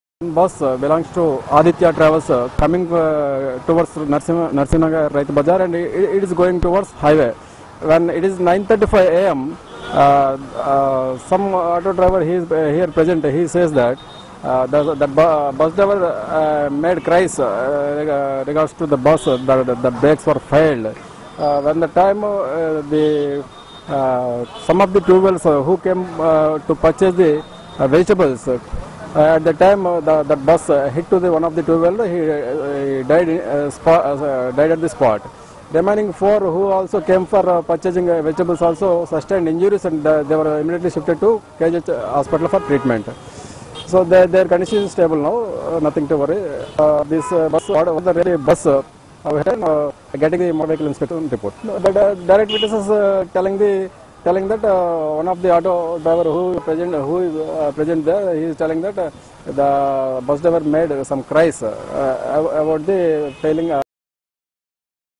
विशाखापत्तनम में दुर्घटना, एक की मौत
रायतू बाजार में बस ने पांच लोगों को कुचला ब्रेक फेल होने से हुई यह दुर्घटना
बस एक निजी ट्रैवल्स की है